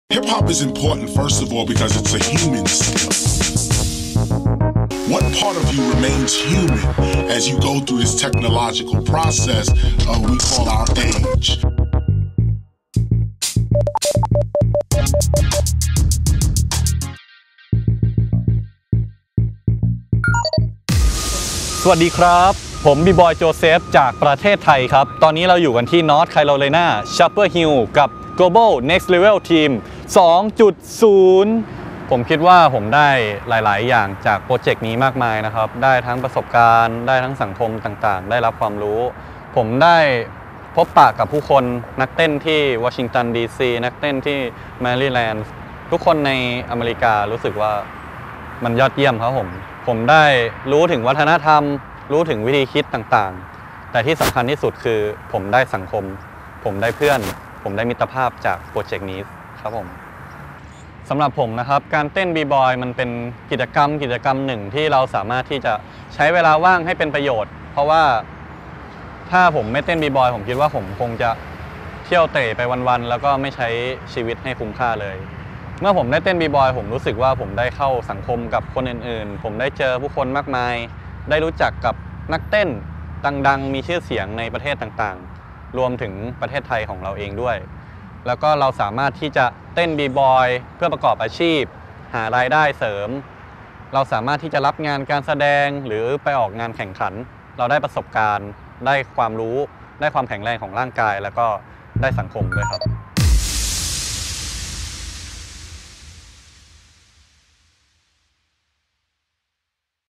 Next Level Global Profiles: B Boy Joseph
-Film by Petna Ndaliko Katondolo of Alkebu Film Productions. Beats by Jaci Caprice.-

Boonyachana Tissakul AKA: Joseph (Double BCR Crew) has practiced dance since the age of twelve, and got serious about battling after winning his division at the first competition he entered, BB Freestyle 2010 in Chiang Rai, Thailand. His crew went on to win the North Thailand Battle of the Year for three years in a row, before the crew dispersed to go to college. Since that time, he has continued to battle and dance throughout Southeast Asia including at Junkfootz on the floor (Chiang Mai), Mae Moe break dance battle (Lampang), and Pa mix battle (Myanmar).